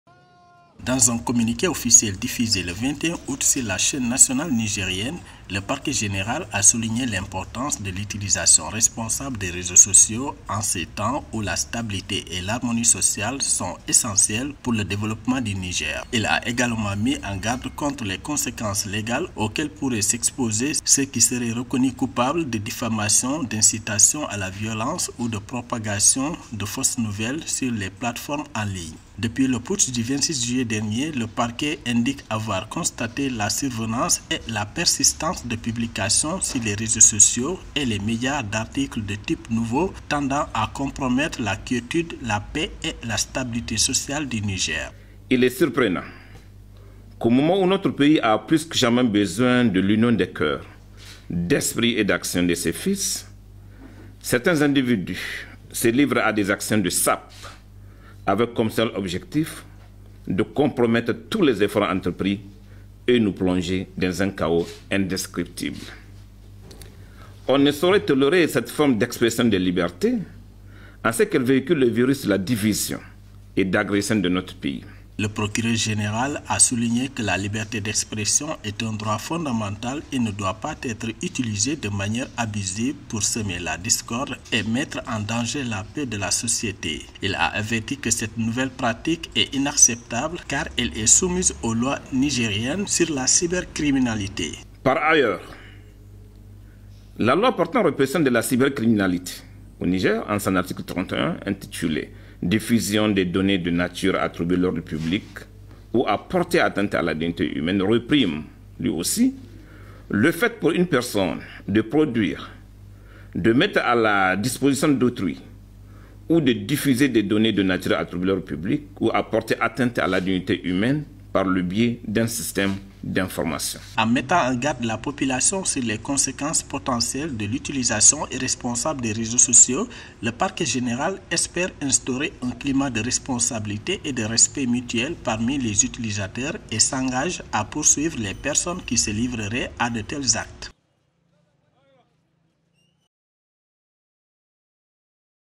Niger : le parquet met en garde contre les publications compromettant la paix sociale
Politique, le parquet général a mis en garde toute personne qui se livrerait à des publications sur les réseaux sociaux et dans les médias compromettant la paix et la stabilité sociale dans le pays, selon un communiqué publié ce lundi 21 août 2023. Dans ce communiqué du procureur général près la Cour d’appel de Niamey, le parquet déplore que depuis le renversement du pouvoir le 26 juillet dernier par des militaires, des individus s’adonnent à des publications sur les réseaux sociaux et des articles compromettant la quiétude, la paix et la stabilité dans le pays.